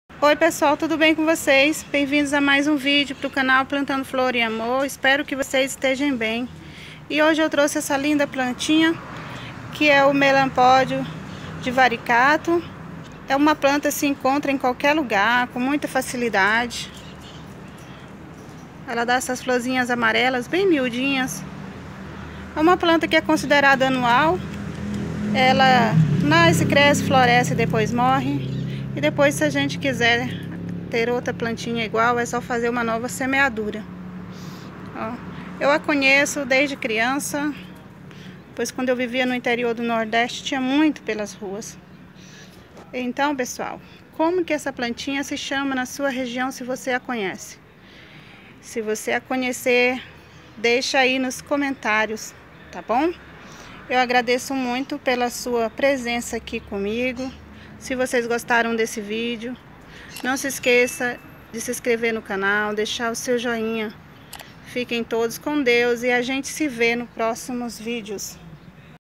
vocês conhecem essa plantinha? | melampodium divaricatum
neste vídeo mostro a planta melampodium divaricatum uma plantinha muito simples mais muito bonita se gostar se escreva no canal deixe seu joinha e compatilha